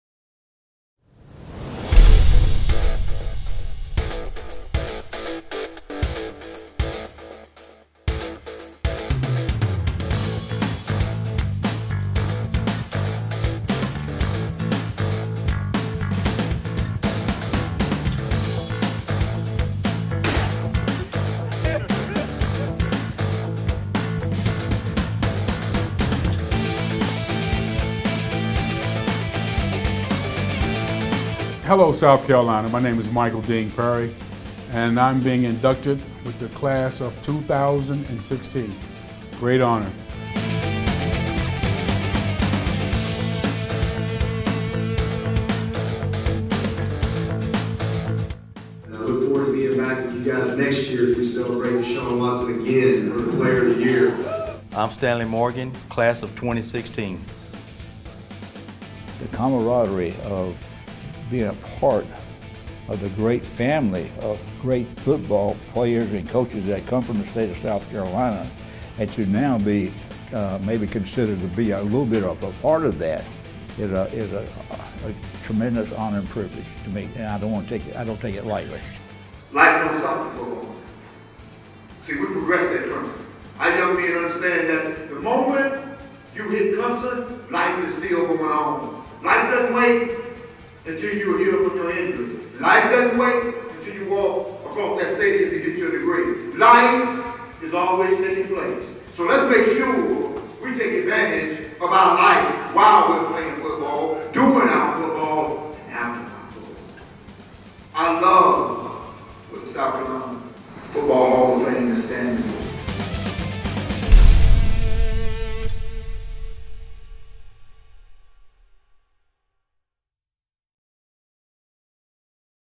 2016 Annual Board meeting at BMW
Review of the 2016 SCFHOF Golf or Gun & Induction Ceremony for our Board of Directors meeting. Created by Bryan Tankersley and Jeter Rhodes of Dark Corner Films.